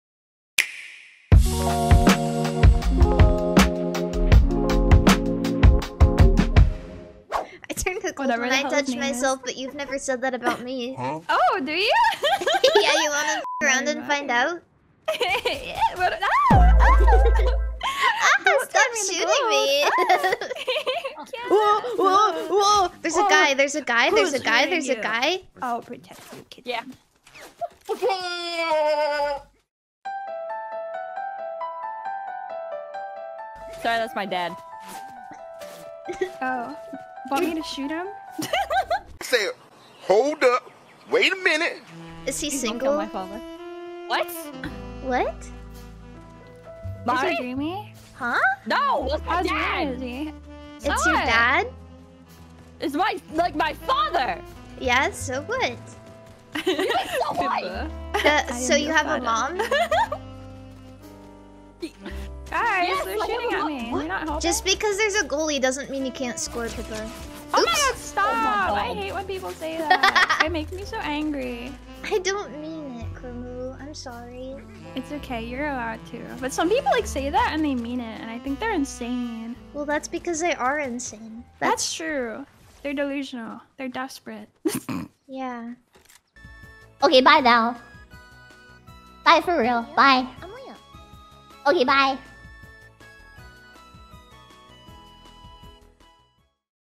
Papakin invades stream and Marimari_EN shoots her shot
the three goobers are at it again